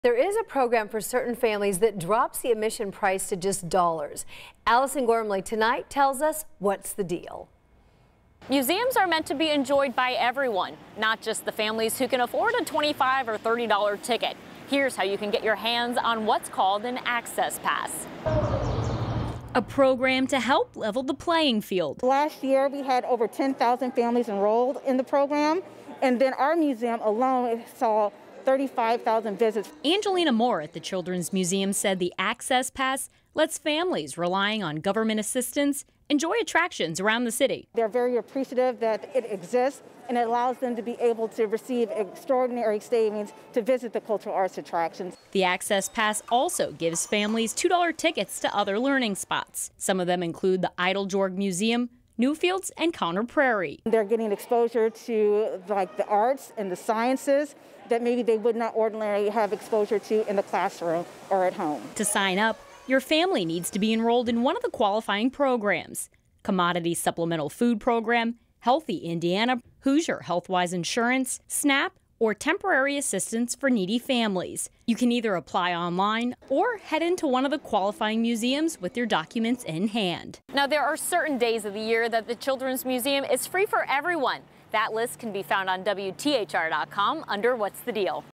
What's the deal with reduced admission prices?
The Children's Museum offers a program, The Access Pass, where qualifying families can get admission for a reduced price.